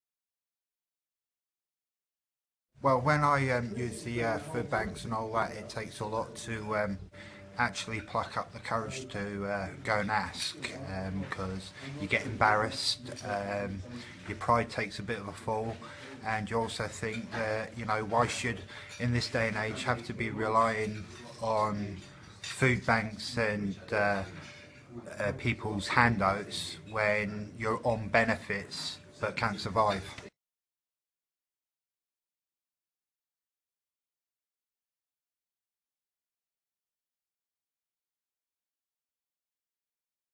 Voices from the Margins Hull clip 1
"In this day and age, why should I have to be relying on food banks and people's handouts?"
People in Hull are proud of their city but feel more should be done for its poorest residents.
Real voices. Real stories. Real democracy.